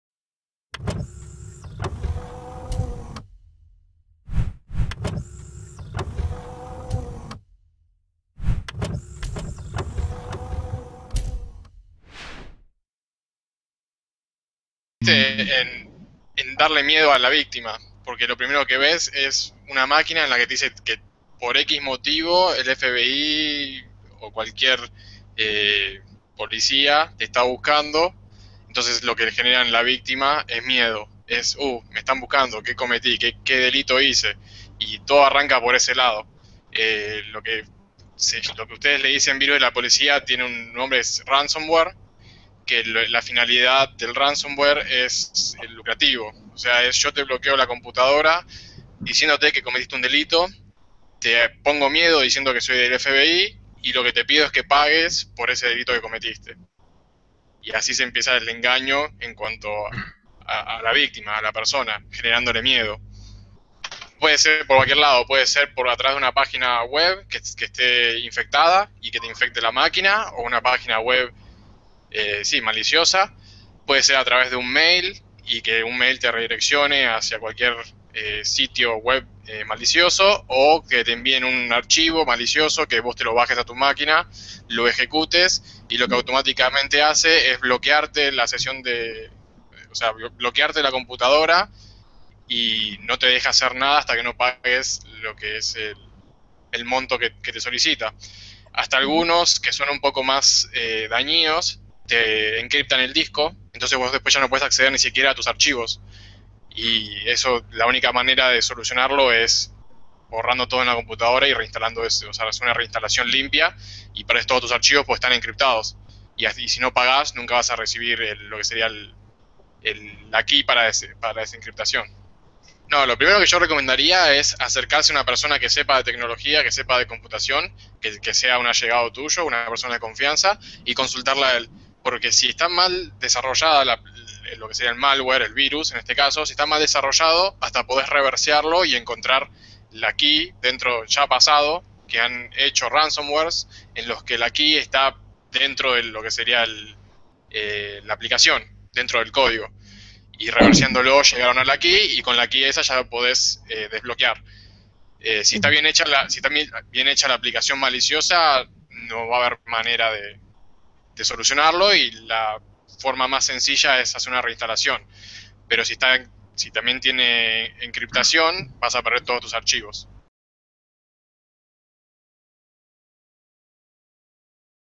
Qué es el virus de la policía y cómo actuar (ransomware)
¿Sabes en qué consiste el virus de la policía? Pues en este momentazo HangoutON, Cristóbal del Pino consultor en seguridad informática, lo explica paso por paso y cómo debes actuar si te ves afectado por este ransomware.

Si quieres ver la charla completa "Estafas en Internet, ransomware y otros malware" este es el enlace